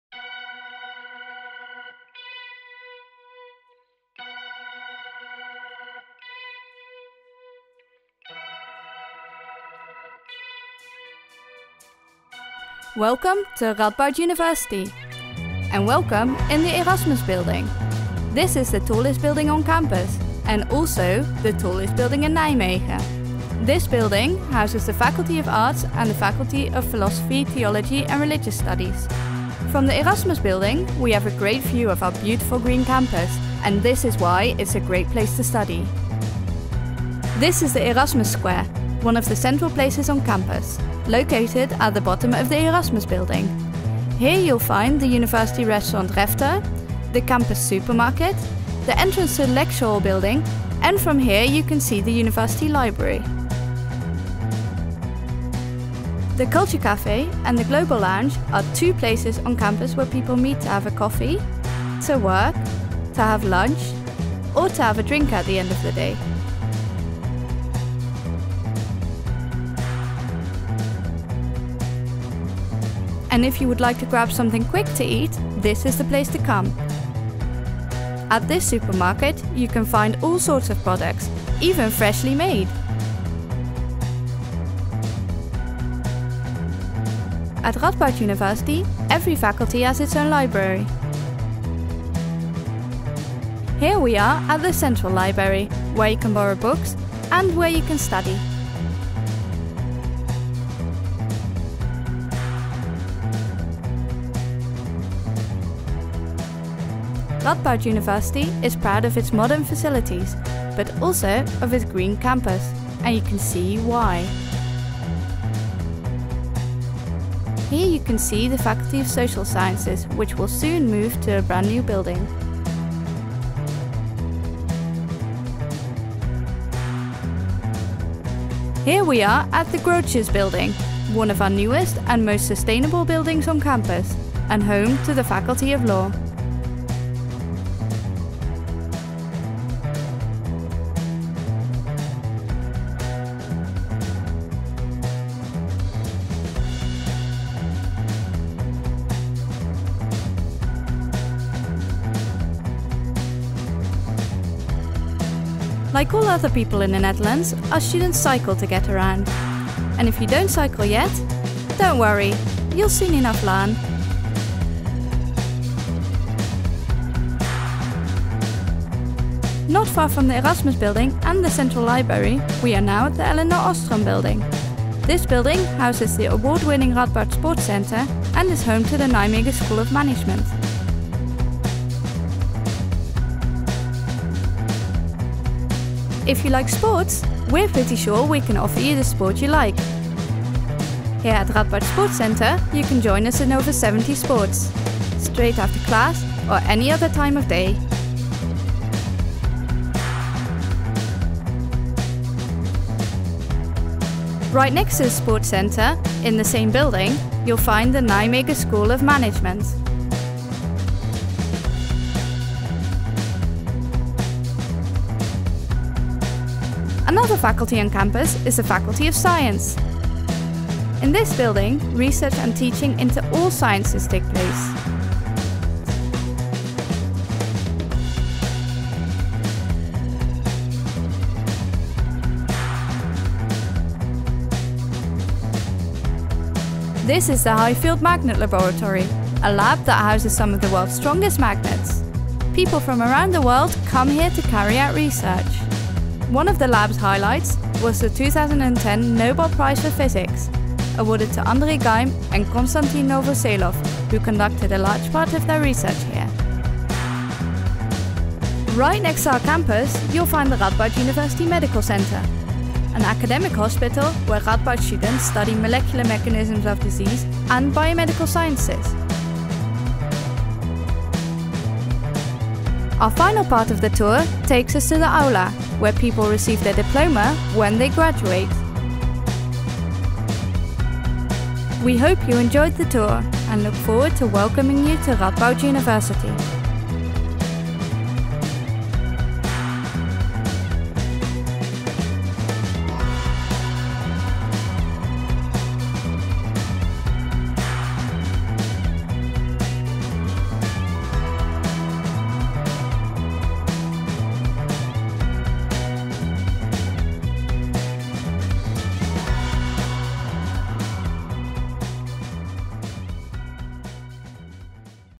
Radboud University Virtual Campus Tour (360 VR video)
Follow a guided VR tour around the Radboud Campus. See our faculties, facilities, students and beautiful green surroundings.